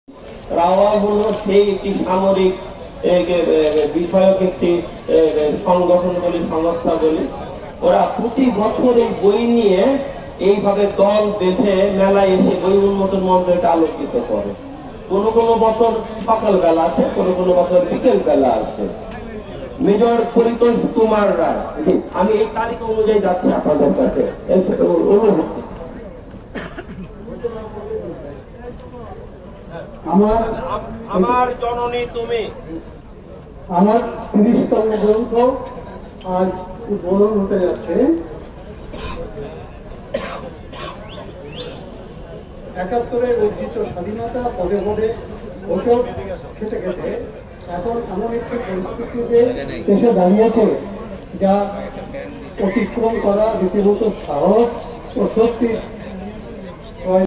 Ekushe Boi Mela Book Unwrapping Ceremony of RAOWA writers on 18th February 2024.
It was a wonderful opportunity to be on the stage at Ekushe Boi Mela Book Unwrapping Ceremony of RAOWA writers on 18th February 2024 where DG Bangla Academy was Chief Guest.